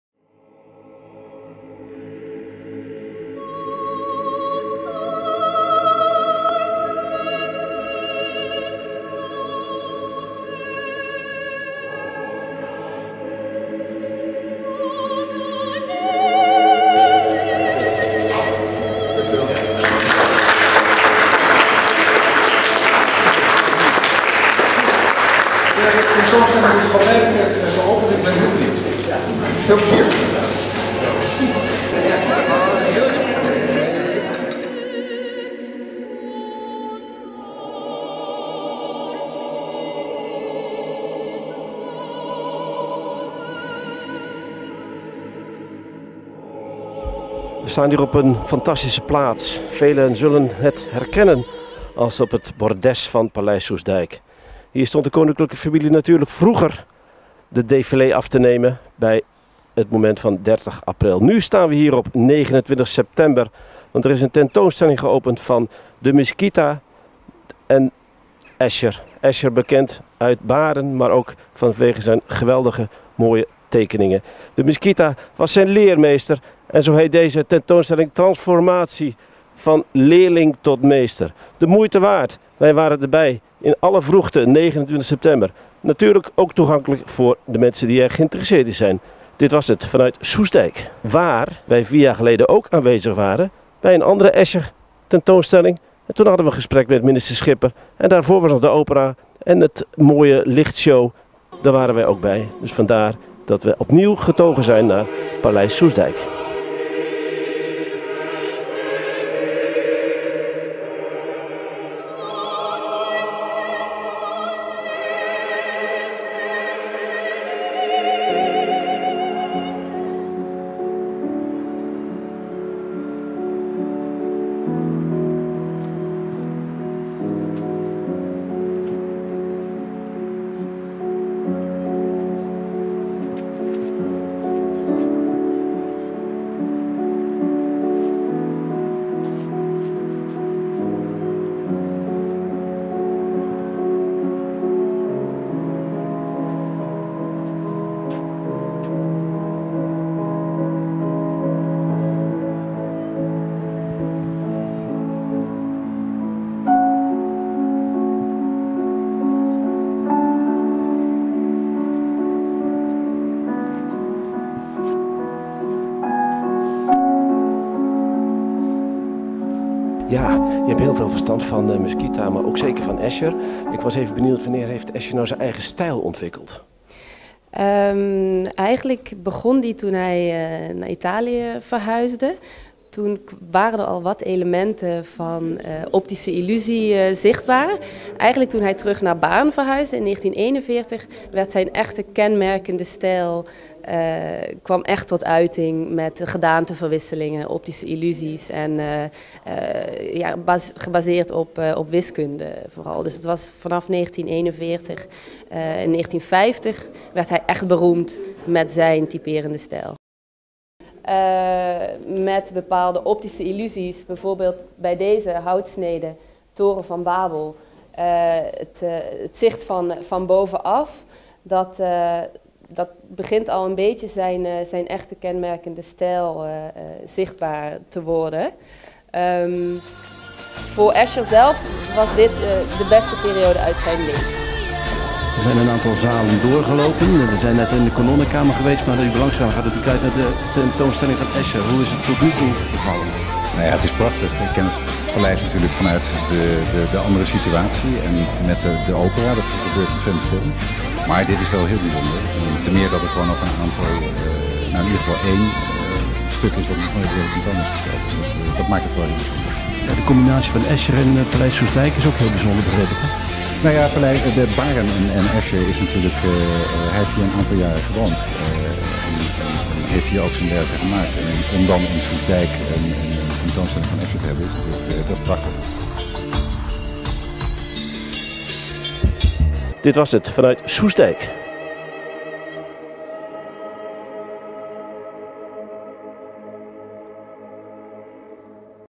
Escher tentoonstelling op paleis Soestdijk
Op paleis Soestdijk gingen wij van RTVBunschoten kijken naar de tentoonstelling:  Transformatie; van leerling tot meester.  M.C. Escher en De Mesquita.